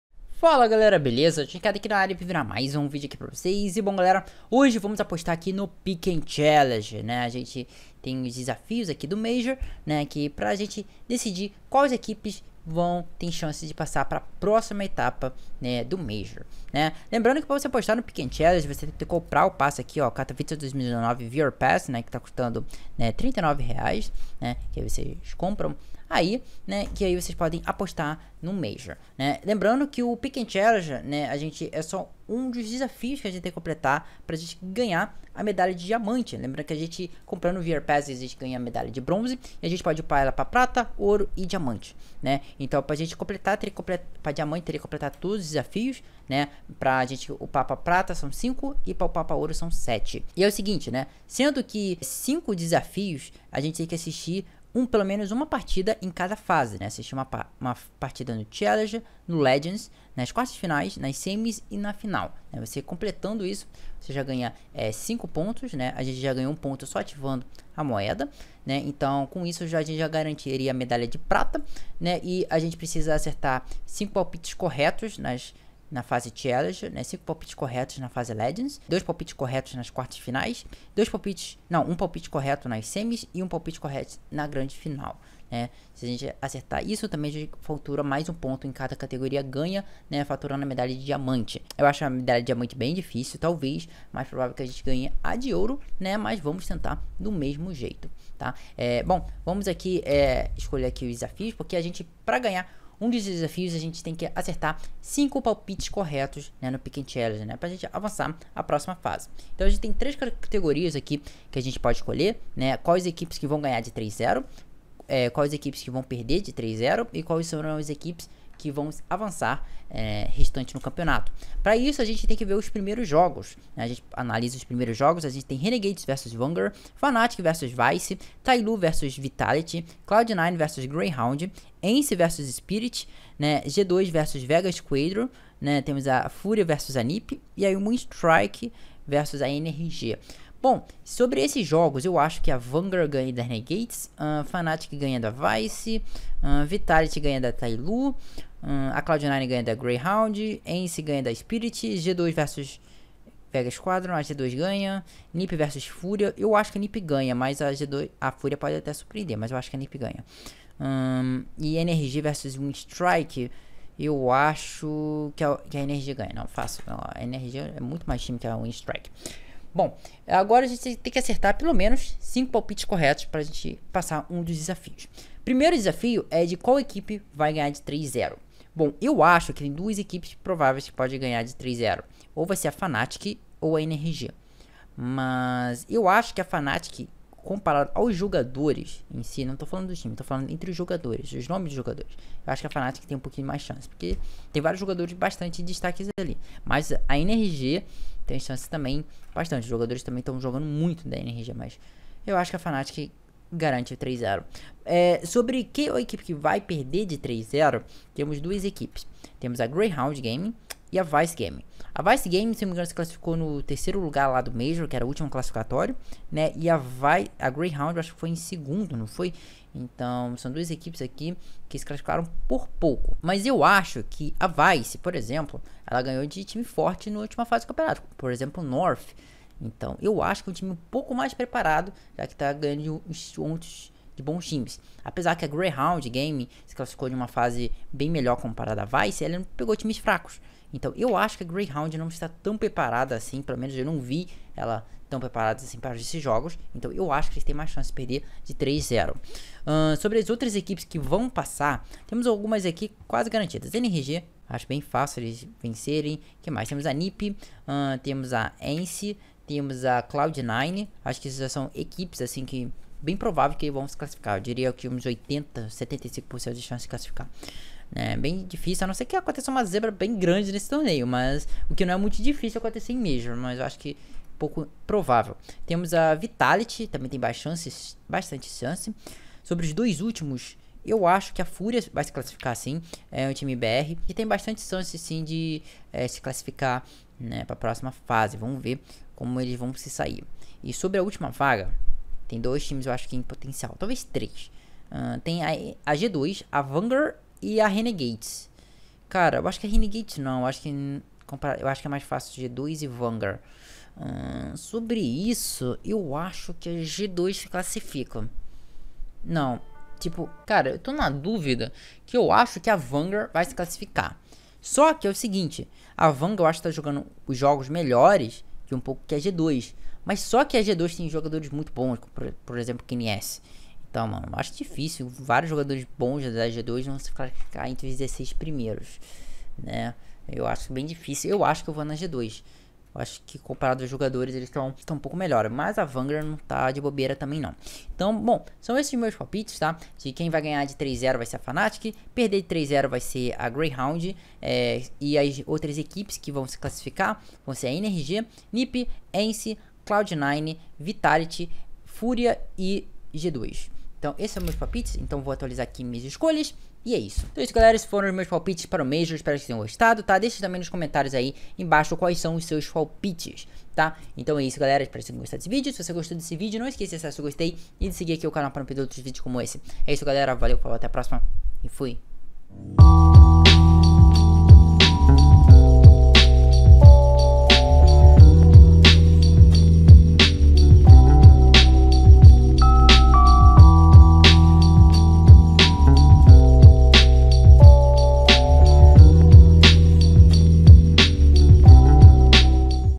FÚRIA VAI GANHAR DA NIP? - PICK'EM CHALLENGE MAJOR KATOWICE 2019
Galera nesse vídeo eu mostro para vocês o Pick'Em Challenge do major Katowice 2019 e faço as minhas apostas, espero que gostem.